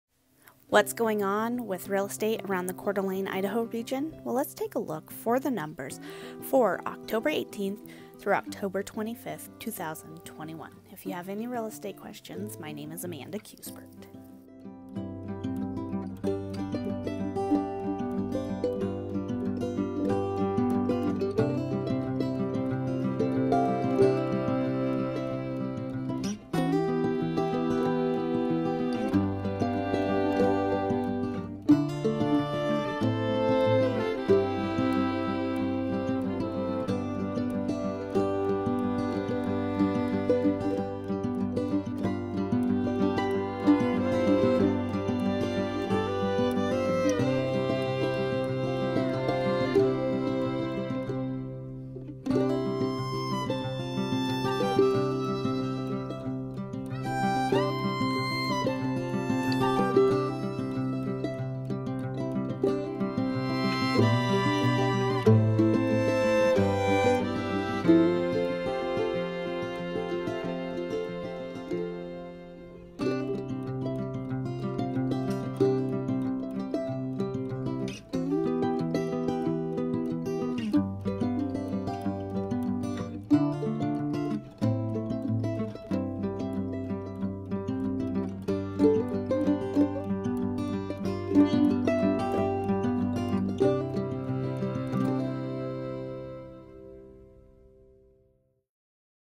Coeur d’ Alene, Idaho Regional Real Estate Market update 10/18-10/25/2021
Here are the latest stats for what is going on in the real estate market for the Coeur d’ Alene, Idaho region. These numbers are for new listings, price changes, pending sales and closed properties for both residential and vacant land.